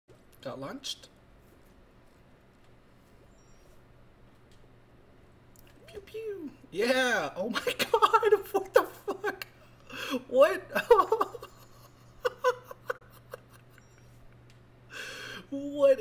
What A Slide
If you do the Superman and hit the corner of the boxing glove, you can get a super slide.